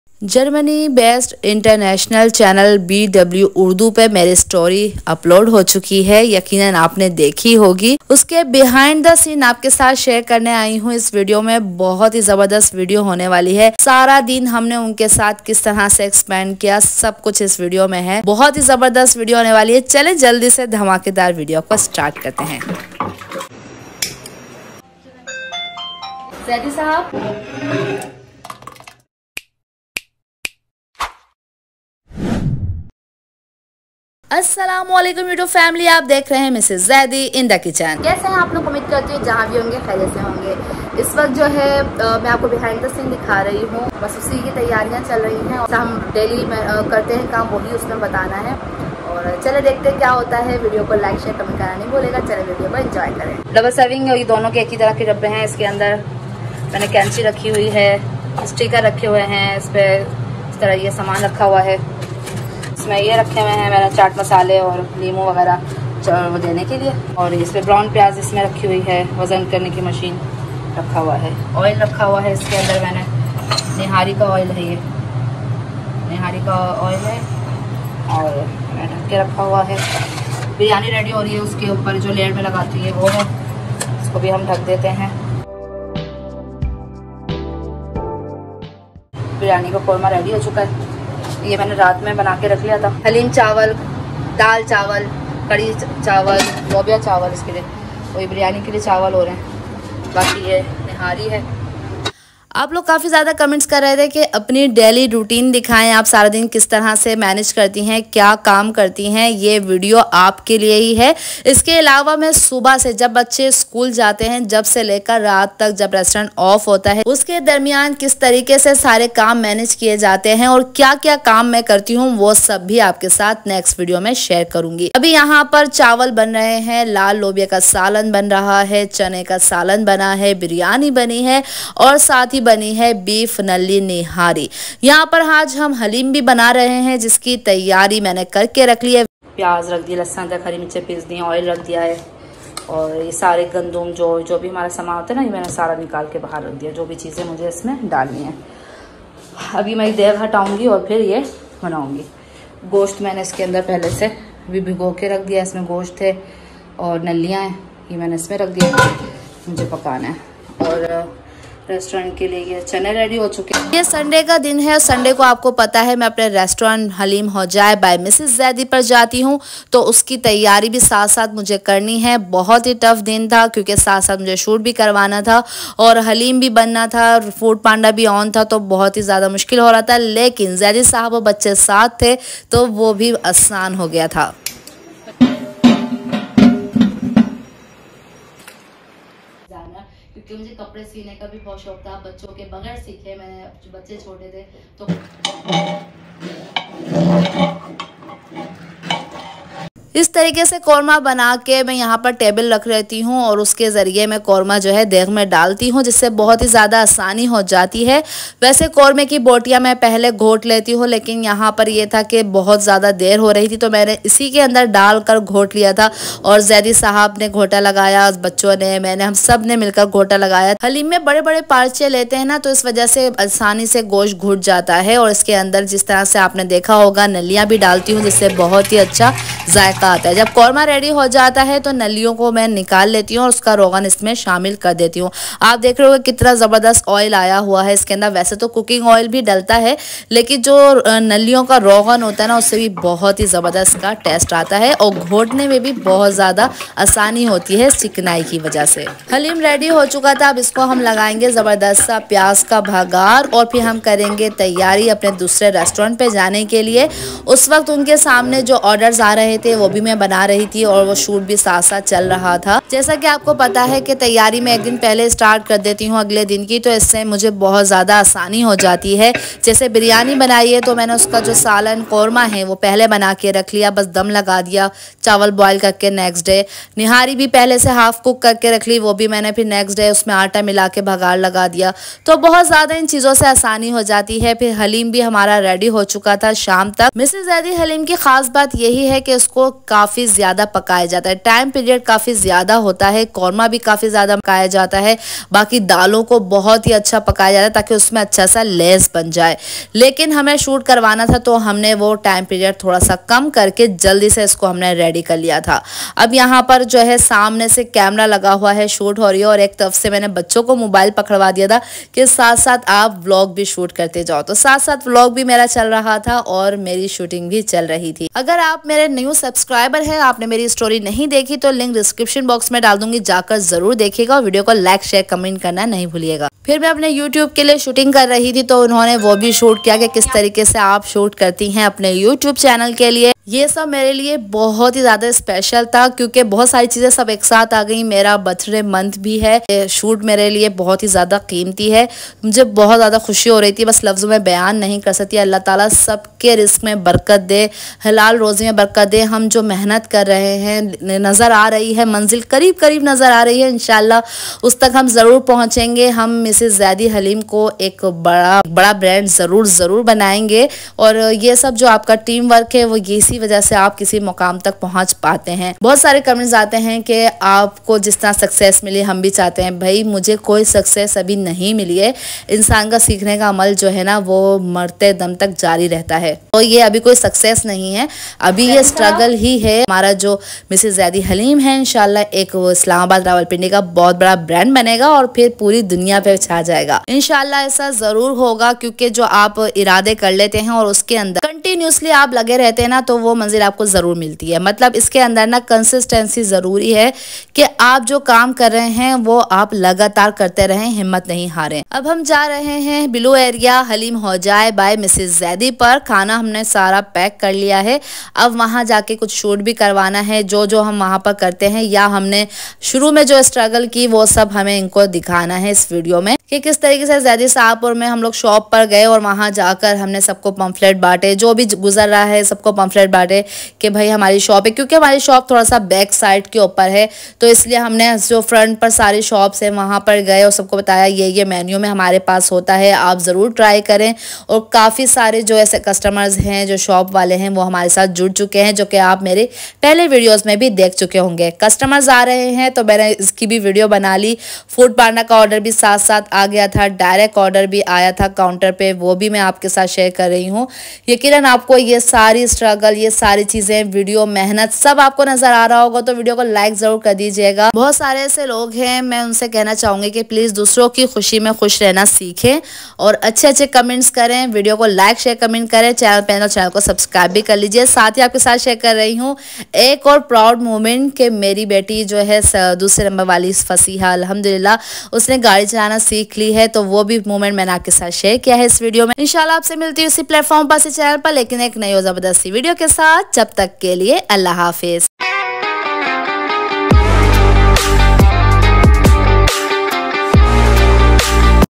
DW Urdu Interview BTS | Mrs. Zaidi Haleem Ki Story| Business Journey Behind the Scenes
"Is video mein dekhiye kaise DW Urdu channel ne hamara exclusive interview shoot kiya, jahan humne apni zindagi aur 'Mrs. Zaidi Haleem' ka safar share kiya. Ye behind-the-scenes (BTS) video aapko dikhayegi hamare interview ke pichhle lamhat aur hamare karobar ki kahani kis tarah se duniya tak pohnchi. Agar aap ek entrepreneur hain ya apni kahani duniya ke samne lana chahte hain, toh yeh video aapke liye hai!